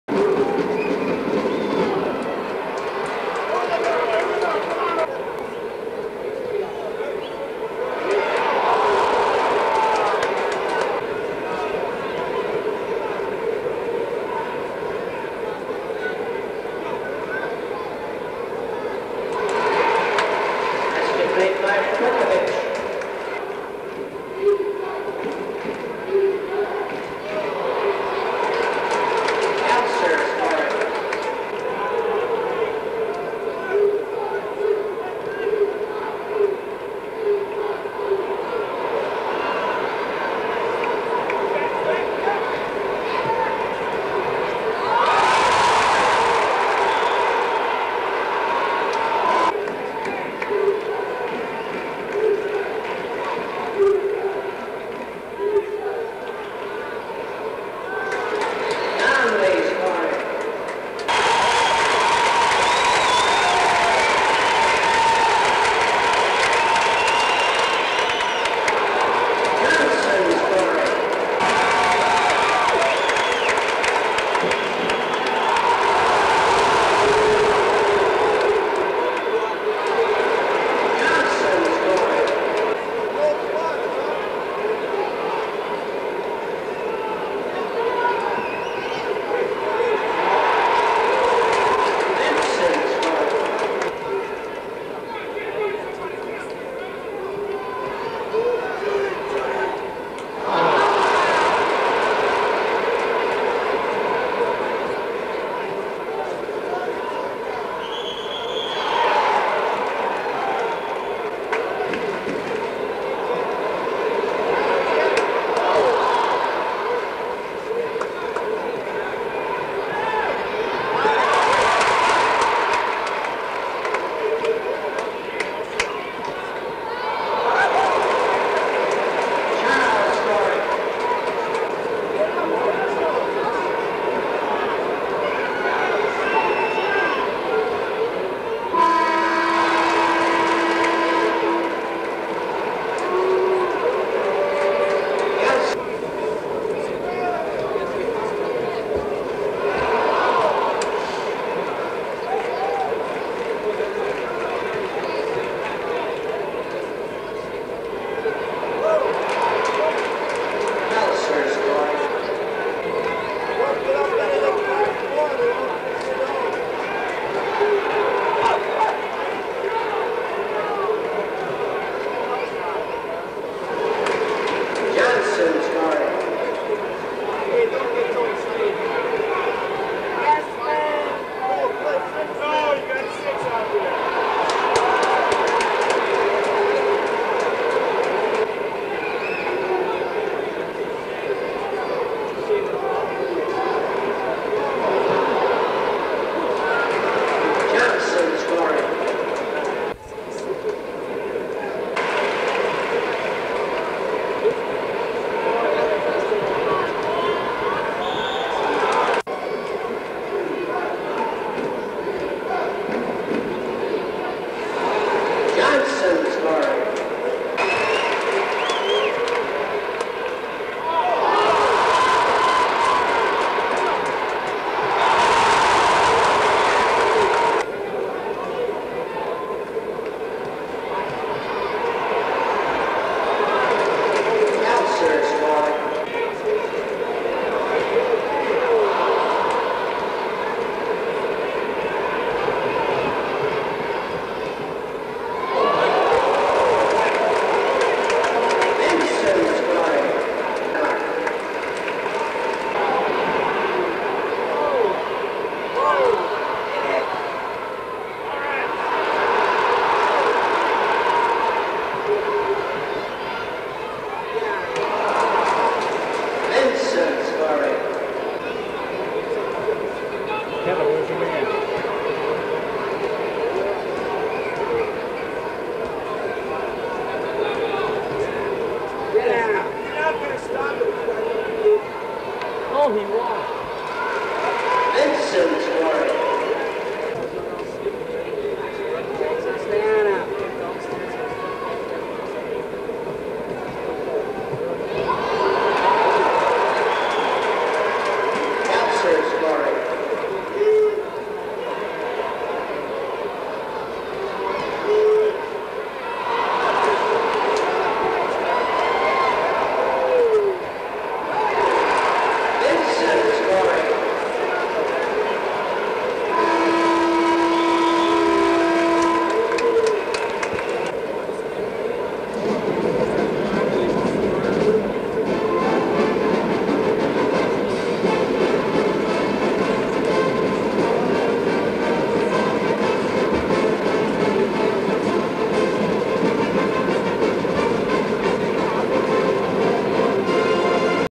3/1/1979 - Michigan State 76 Minnesota 63
The Spartans defeat the Golden Gophers by 13 at Williams Arena.  Jay Vincent leads MSU with 25 points and Greg Kelser leads MSU with 10 rebounds.